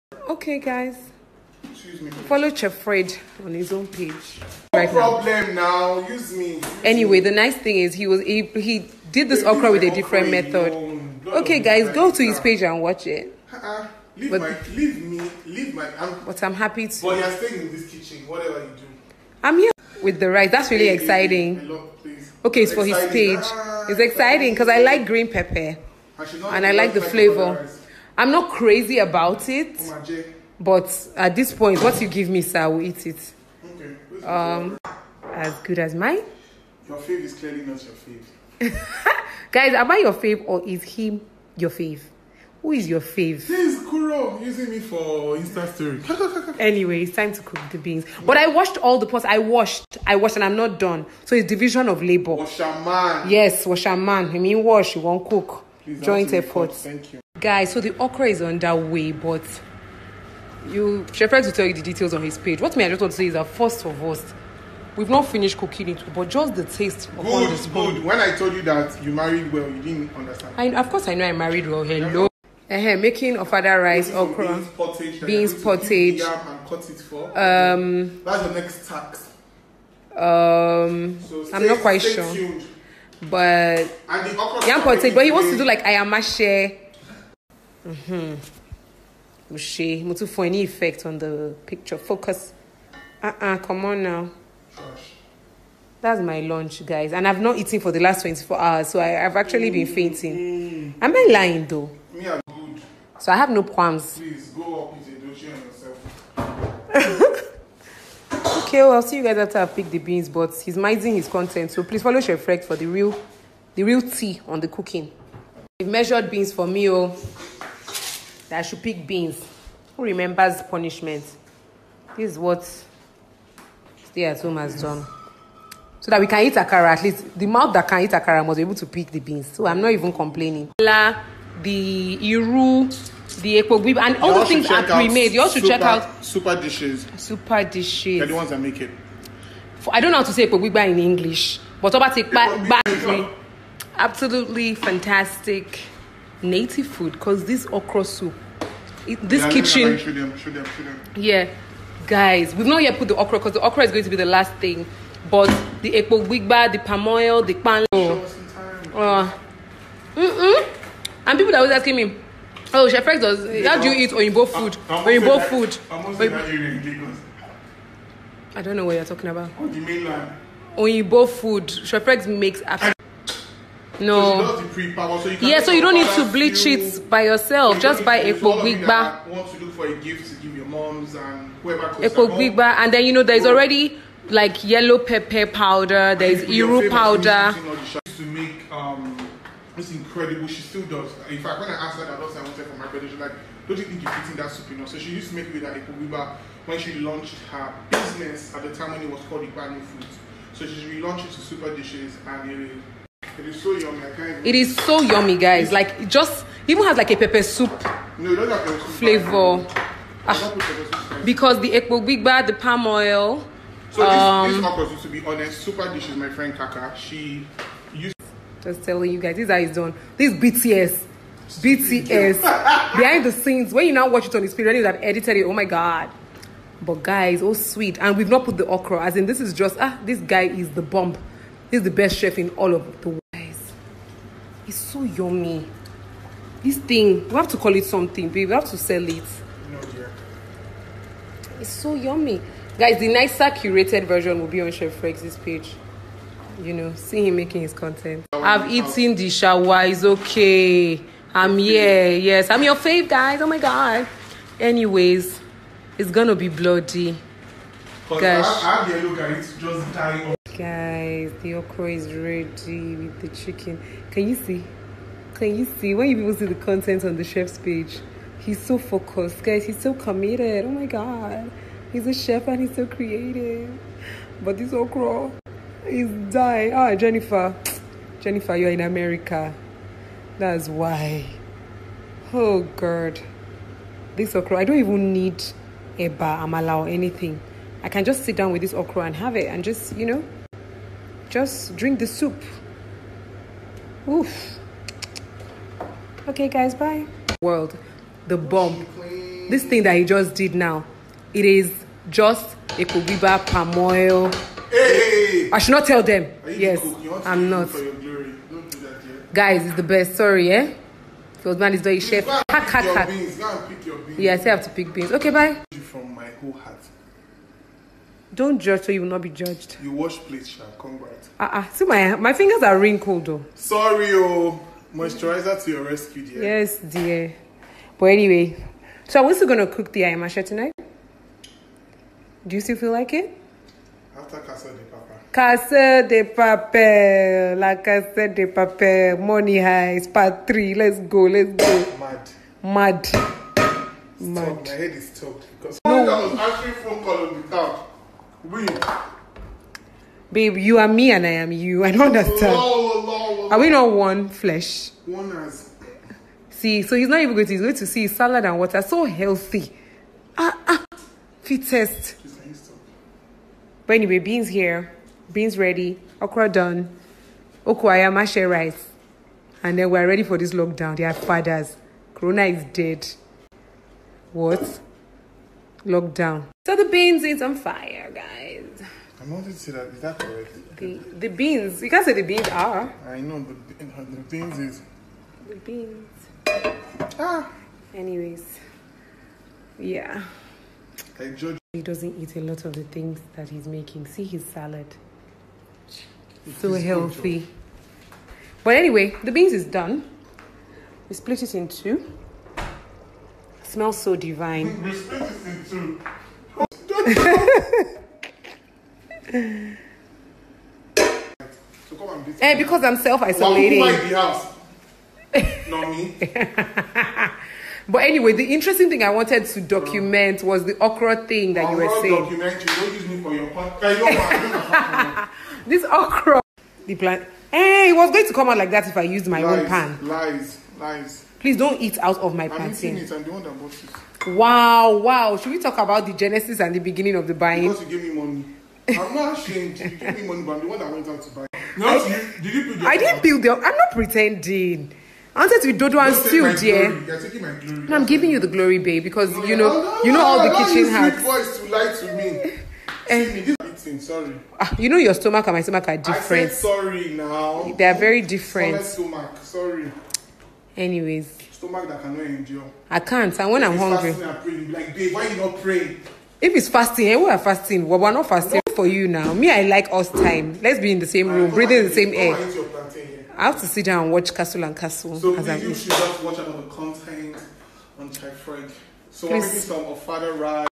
Chef Fregz & Lala Akindoju - Kitchen Banter ... Beans Okra Plaintain
Newly Weds Chef Fregz & Lala Akindoju - Kitchen Banter ... Beans Okra Plaintain

Jaiyeorie.blogspot.com

🎥 Chef Fregz & Lala Akindoju ig